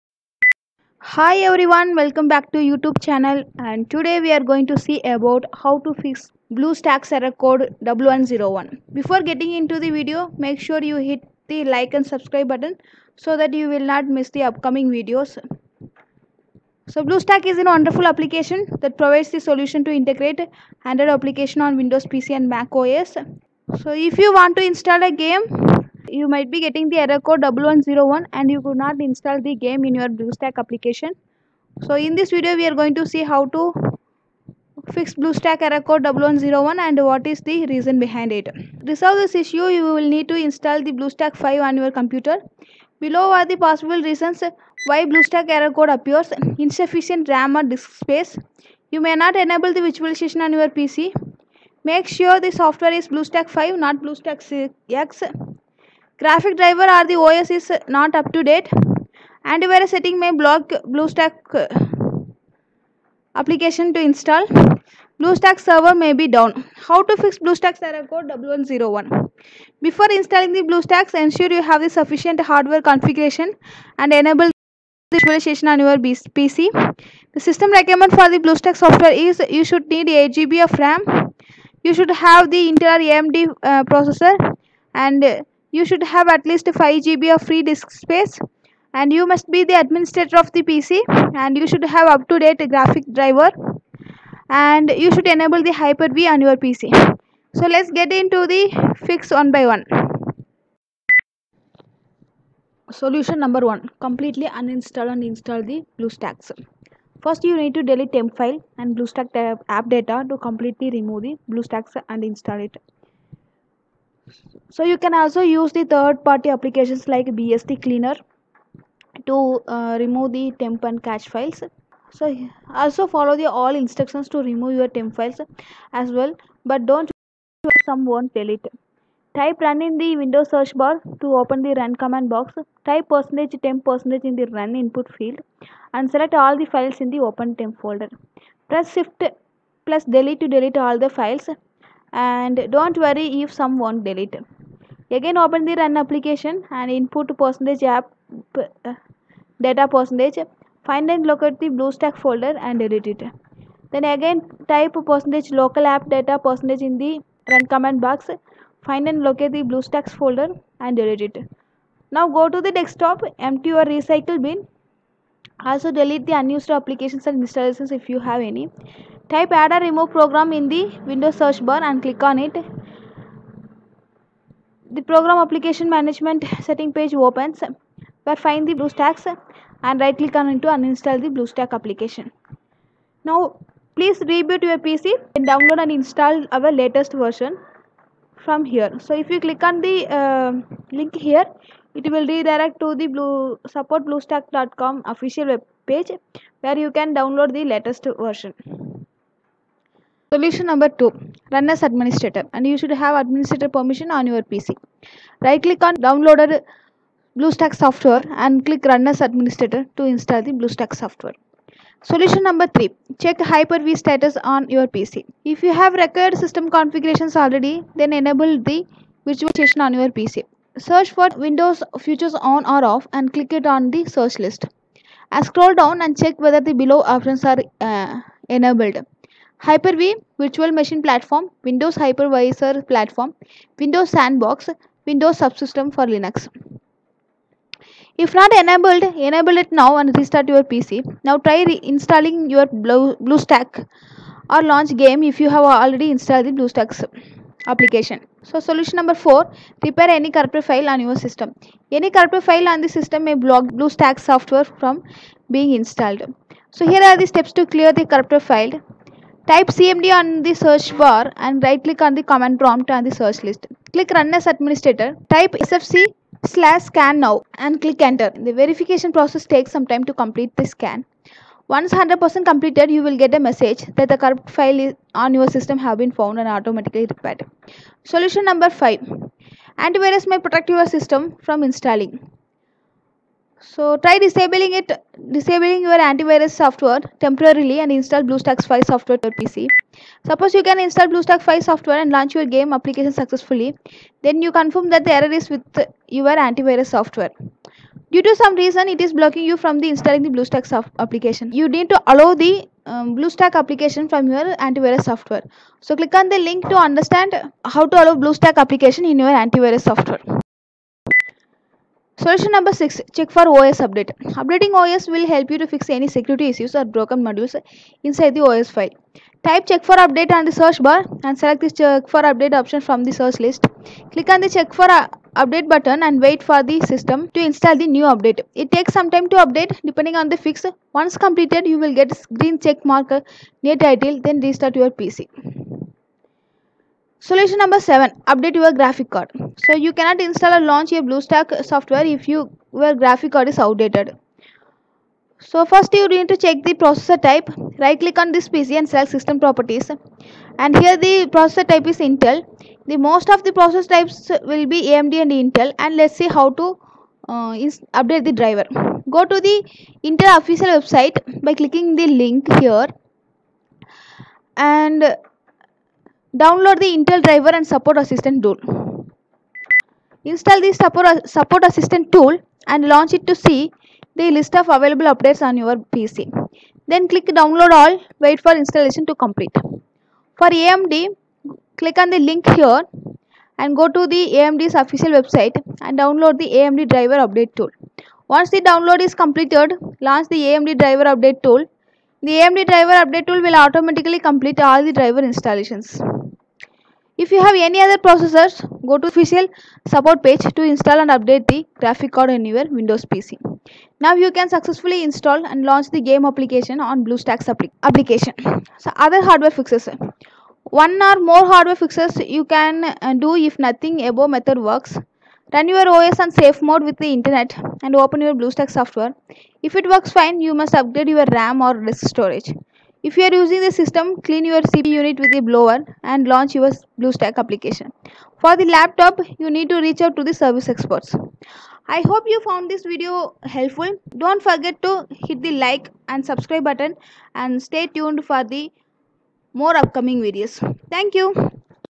How To Fix BlueStacks Error Code 1101 Something Went Wrong Installation Failed
Hi Everyone! Welcome to our Video. BlueStacks is a wonderful application that provides the solution to integrate Android applications on Windows PC and Mac OS. If you want to experience the Android Application on a large screen with your PC, you can run the Android Application by installing BlueStacks Software. You will be stopped by following the error message when installing a game.
The full error is "Something went wrong” followed by “Installation failed, Error Code: 1101"

0:00 Introduction
0:42 What Is BlueStacks Error Code 1101?
1:28 Solution 1: Completely Uninstall & Install BlueStacks
3:06 Solution 2: Run As Administrator
3:18 Solution 3: Check Hyper V Status On Your PC
3:52 Solution 4:  Repair Any Corrupted File On Your System
4:22 Solution 5: Antivirus May Protect Your System From Installing
4:54 Solution 6: Check For OS Update
5:20 Solution 7: Update Your Graphics Card
6:30 Solution 8: Other Hardware Fixes
6:55 Conclusion